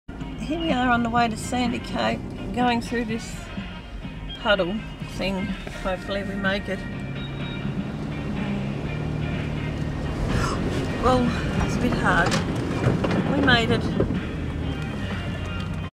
Tasmania always water on the track.mpg
Water holes on the Sandy Cape Heritage Track